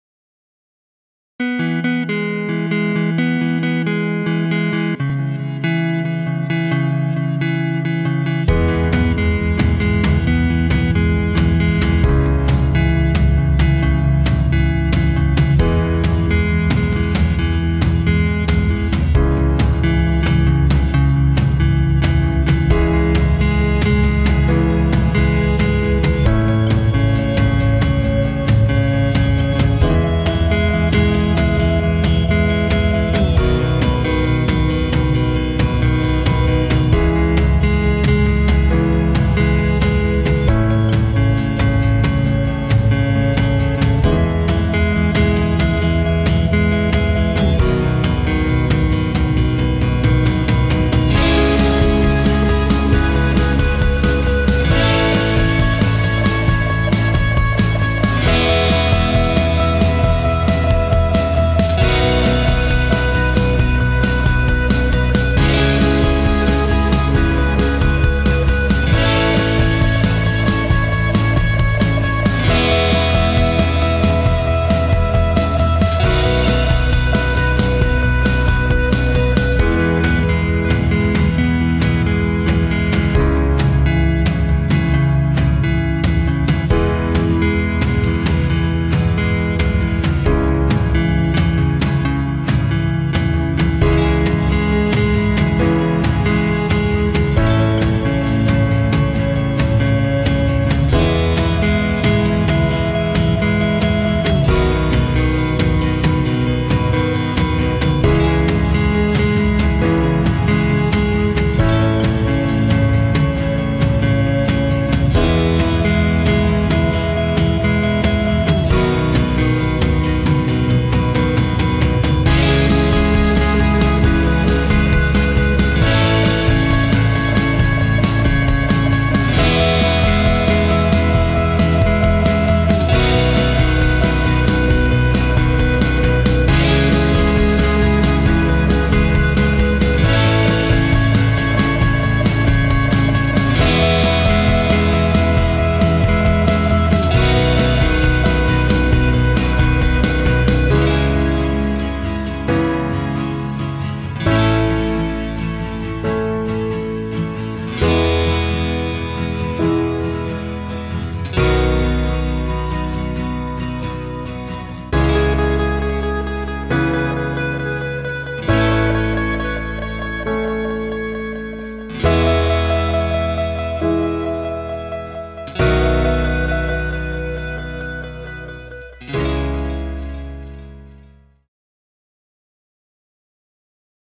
Drone Flights - Spring 2020 - Ontario - DJI Mavic Air + Mini
A collection of drone footage from spring of 2020!